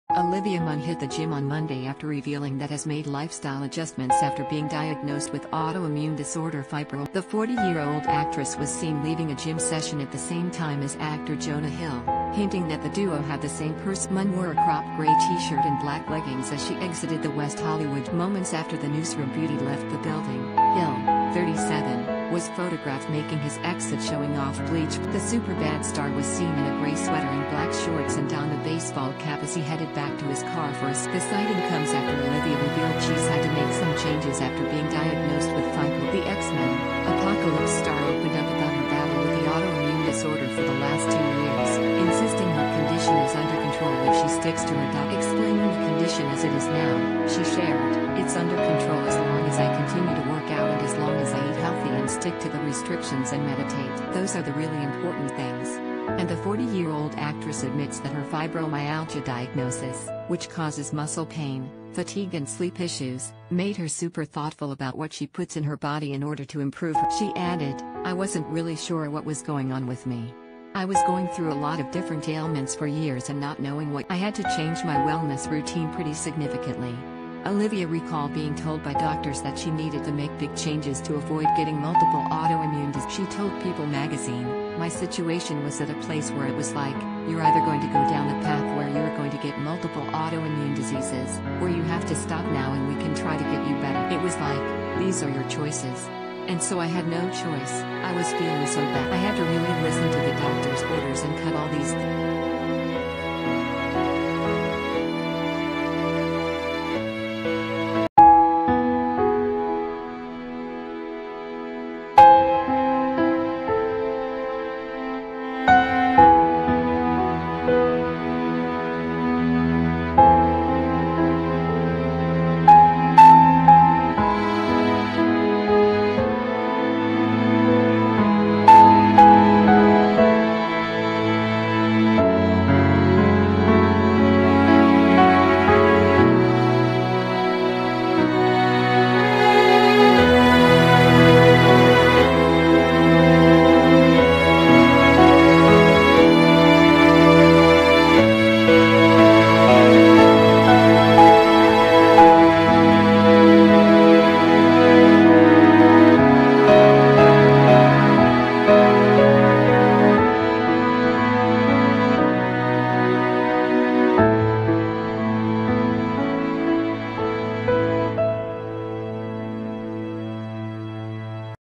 Olivia Munn and Jonah Hill spotting leaving the same gym
Olivia Munn hit the gym on Monday after revealing that has made lifestyle adjustments after being diagnosed with autoimmune disorder fibromyalgia.The 40-year-old actress was seen leaving a gym session at the same time as actor Jonah Hill, hinting that the duo have the same personal trainer.Munn wore a cropped grey T-shirt and black leggings as she exited the West Hollywood gym.    Moments after the Newsroom beauty left the building, Hill, 37, was photographed making his exit showing off bleach blond locks.The Superbad star was seen in a grey sweater and black shorts and donned a baseball cap as he headed back to his car for a cigarette.The sighting comes after Olivia revealed she's had to make some changes after being diagnosed with fibromyalgia. The X-Men: Apocalypse star opened up about her battle with the autoimmune disorder for the last two years, insisting her condition is 'under control' if she sticks to her doctor's advice.    Explaining the condition as it is now, she shared: 'It's under control as long as I continue to work out and as long as I eat healthy and stick to the restrictions and meditate. Those are the really important things.' And the 40-year-old actress admits that her fibromyalgia diagnosis - which causes muscle pain, fatigue and sleep issues - made her 'super thoughtful' about what she puts in her body in order to improve her health. She added: 'I wasn't really sure what was going on with me. I was going through a lot of different ailments for years and not knowing what was happening ... I had to change my wellness routine pretty significantly.    Olivia recalled being told by doctors that she needed to make big changes to avoid getting multiple autoimmune diseases.She told People magazine: 'My situation was at a place where it was like, "You're either going to go down a path where you're going to get multiple autoimmune diseases, or you have to stop now and we can try to get you better.'It was like, "These are your choices." And so I had no choice, I was feeling so bad. I had to really listen to the doctor's orders and cut all these things out.'